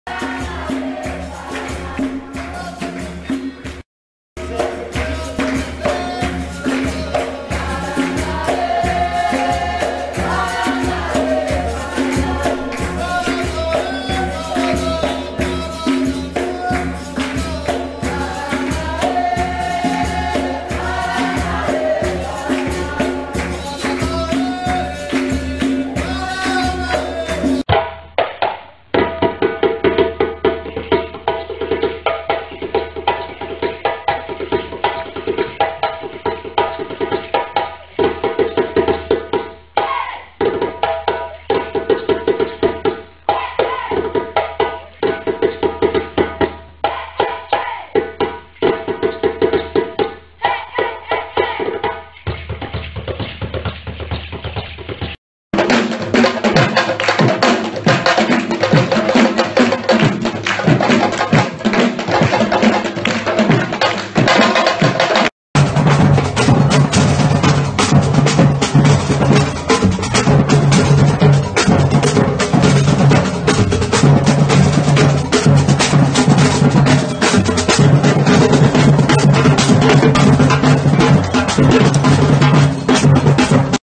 percussions et danses au festival Jazz à Oricourt
Entre les concerts au Festival Jazz à Oricourt 3
le samedi 8 septembre 2018 au Château fort d'Oricourt :
Percussions et danses !
avec Ritmo da Capoeira (capoeira), Boloföli (percussion et danse africaine), Madness(hip-hop) et la Batou Bled'ARTS (batucada)